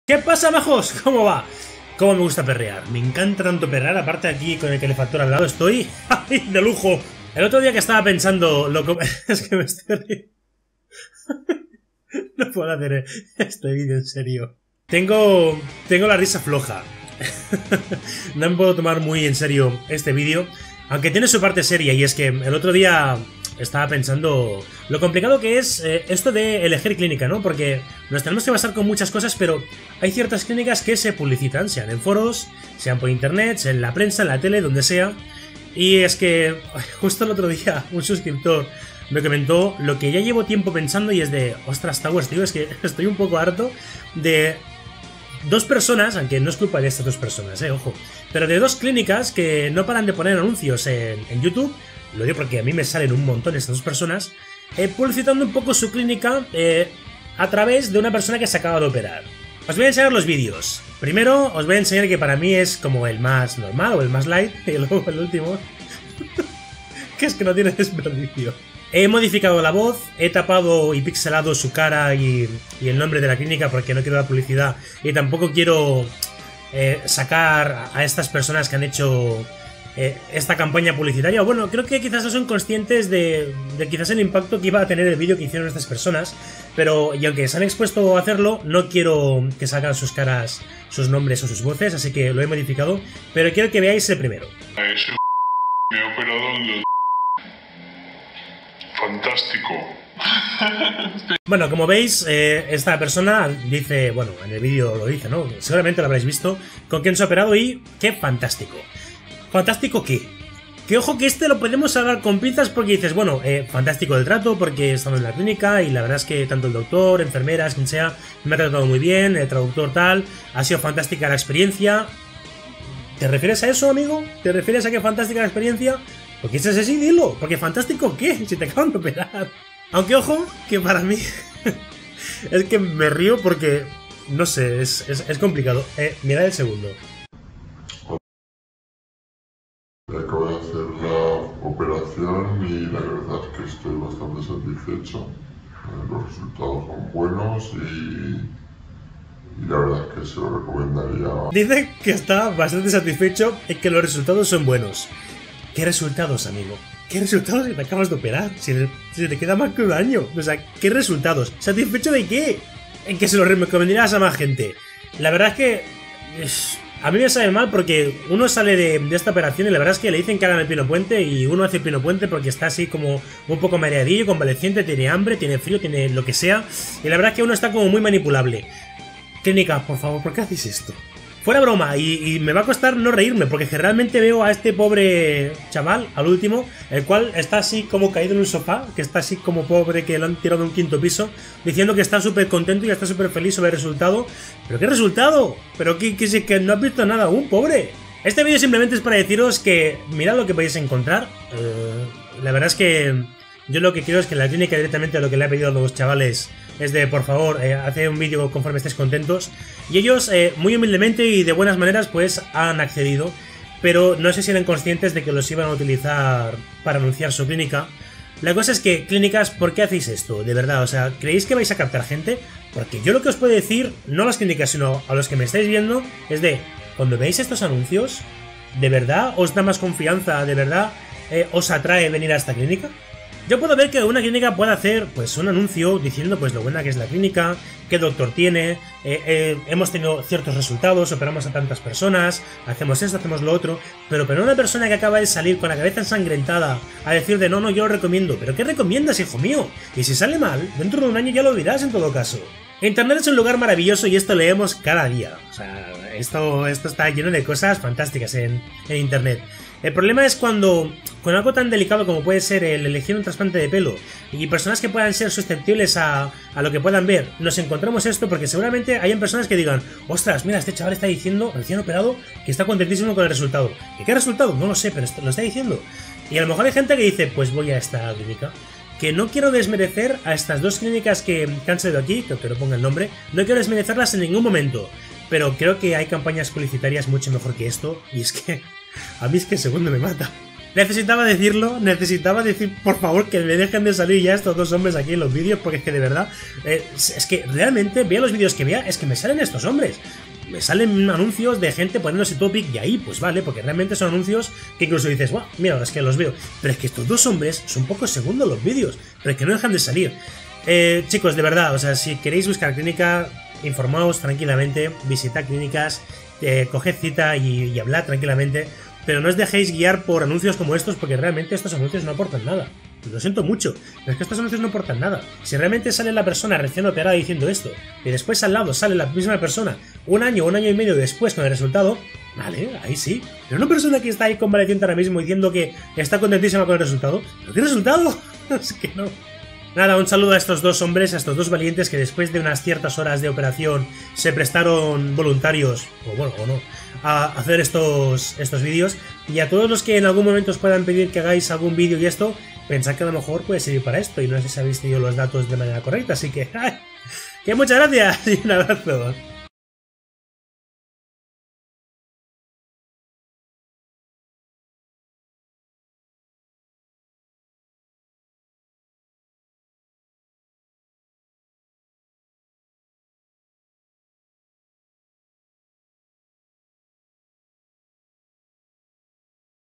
ANUNCIOS de CLÍNICAS de TRASPLANTE DE PELO
Clínicas, por favor, no hagáis estos anuncios, por dios!

Seguro que habéis visto estos dos anuncios!

Servidor Pelowers!